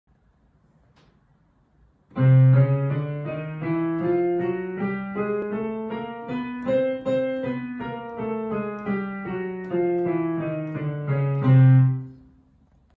Soft Mozart Lebanon: Amber plays the chromatic scale. Preparatory level C
Teacher: Lara Molaeb Amber started studying the piano with Mrs. Lara Molaeb in January 2021, 2.5 months ago